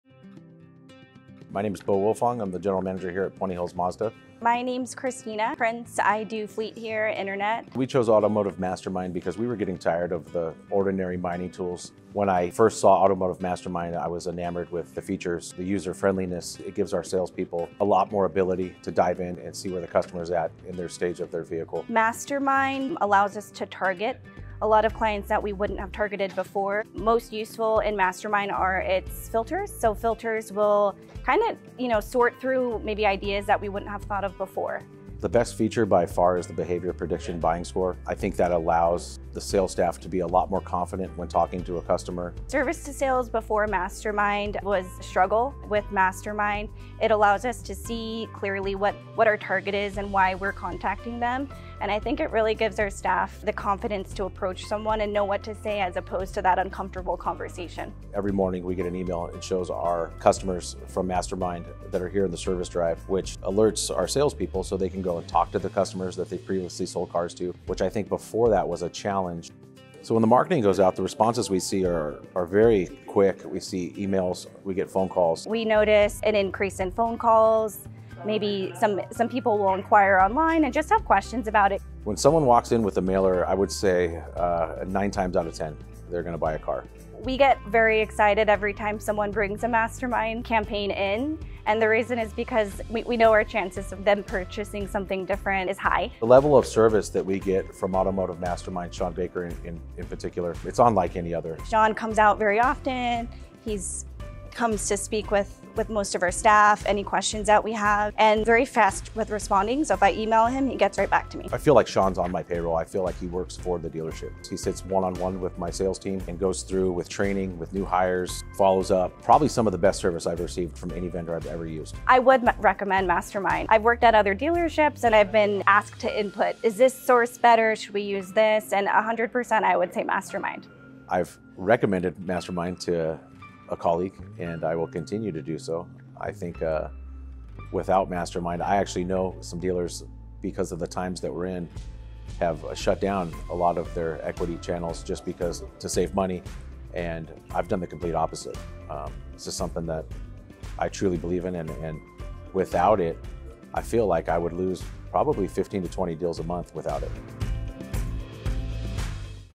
automotiveMastermind Dealer Testimonial | Puente Hills Mazda
Hear from the team at Puente Hills Mazda. Thanks to Mastermind's user-friendly features and filters, the Sales staff can easily target prospects that they would not have identified as opportunities before.

"Without Mastermind, I feel like I would probably lose 15 to 20 deals a month." - Bo Wilfong, General Manager, Puente Hills Mazda.

Are you looking to increase your customer retention rate, convert service to new car sales and acquire more pre-owned inventory? Request a free demo today.